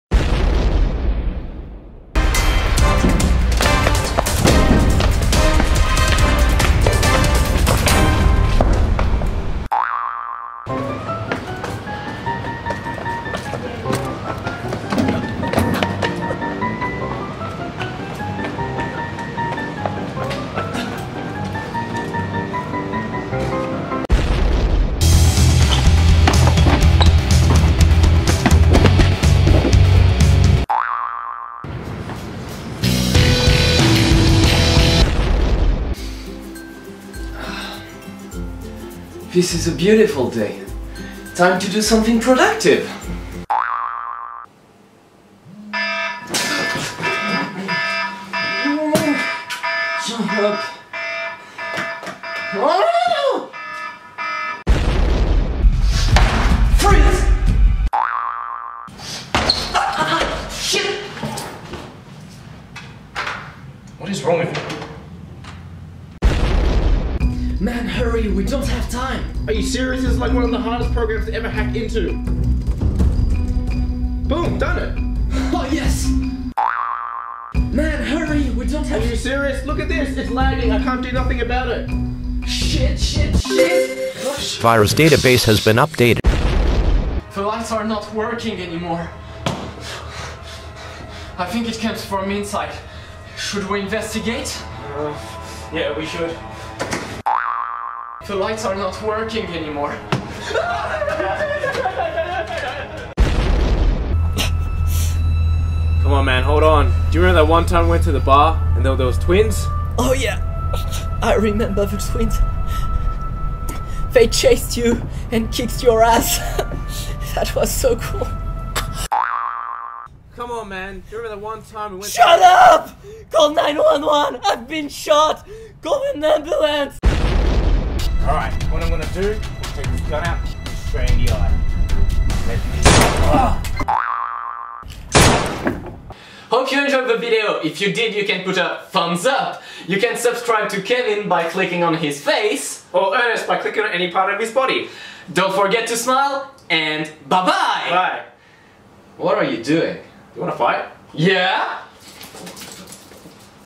MOVIES VS REALITY
Movies can be pretty awesome so I asked myself what those scenes would look like if it was in real life and the result is. a bit less epic... Ok it's crap =D

Don't forget to go check his channel he's EPIC!